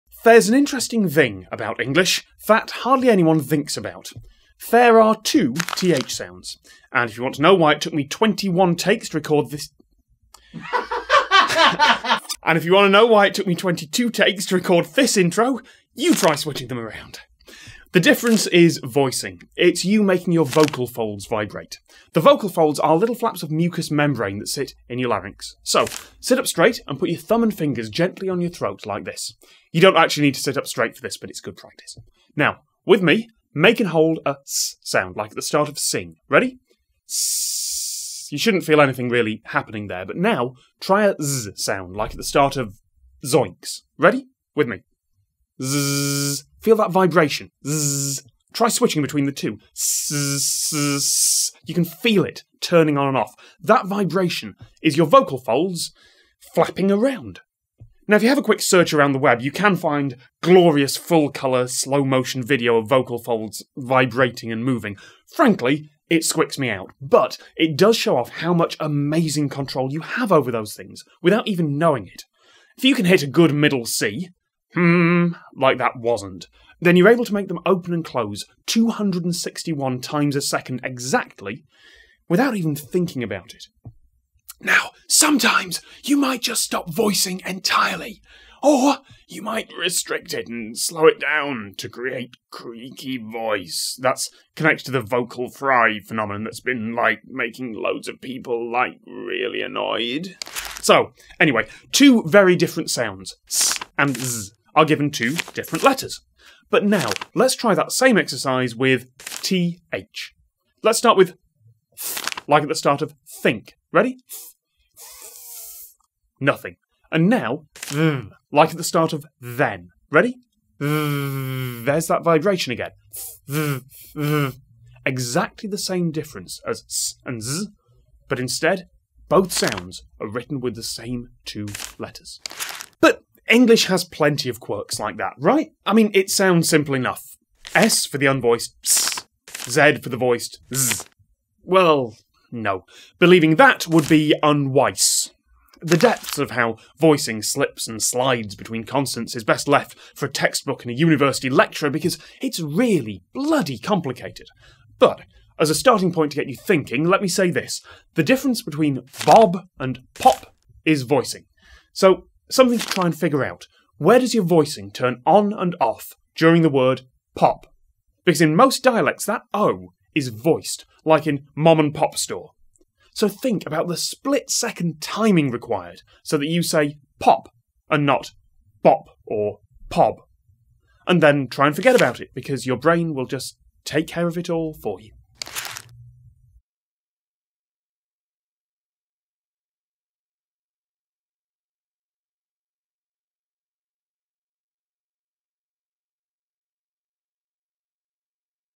Making Flaps Vibrate In Your Throat: Voicing
- @tomscott - There's an interesting thing about English that hardly anyone thinks about. There are two "th" sounds. And if you want to know why it took me twenty-one takes to record this intro, you try switching them round.

Filmed at YouTube Space, London